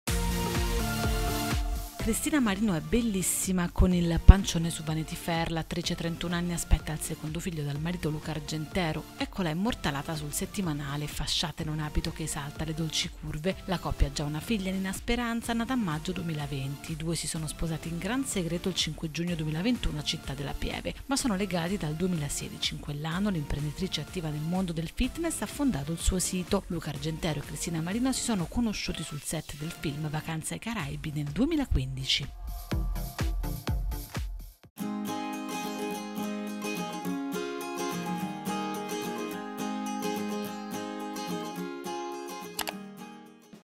Cristina Marino splendida col pancione su Vanity Fair: Luca Argentero sarà papà Bis
Cristina Marino è fiera del suo corpo che cambia. Incinta per la seconda volta, posa per Vanity Fair: la prima foto col pancione la immortala sensuale e bellissima con indosso una tutina aderente che mette in evidenza le sue forme da mamma. Sarà il suo secondo figlio insieme al marito Luca Argentero.